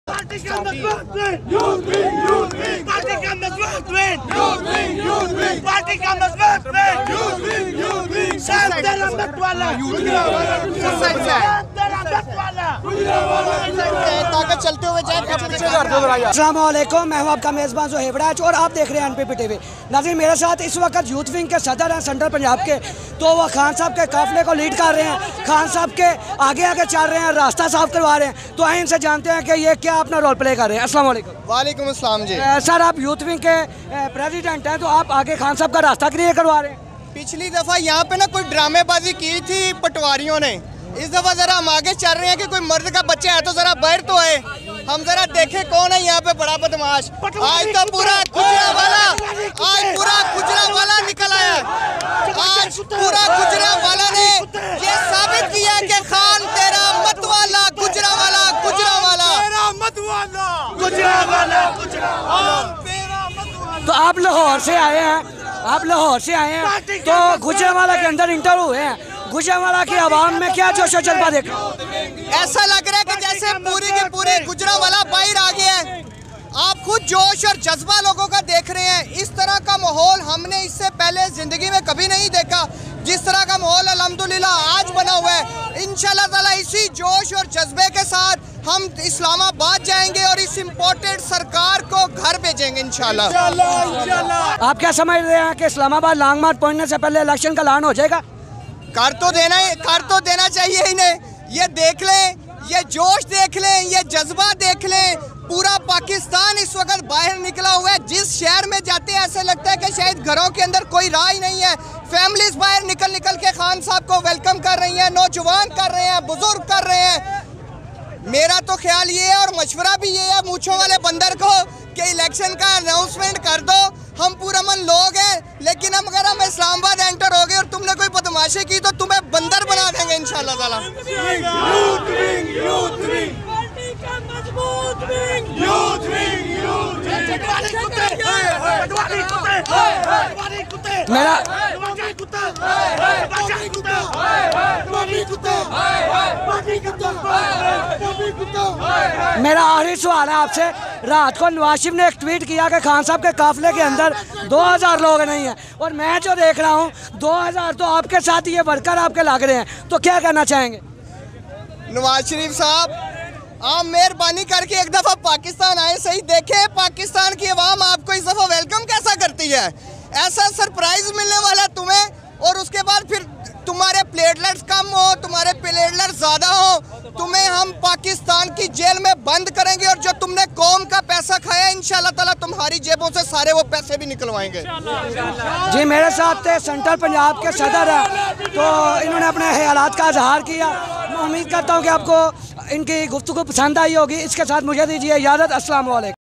youth Wing sadar stand for front line for protect khan ||NPPTV
Asslam-o-Alkum.viewer's this video is related pti youth Wing  sadar in front line and stand for Imran khan.he says tells patwari we are with Imran khan in last spot for blood. NPPTV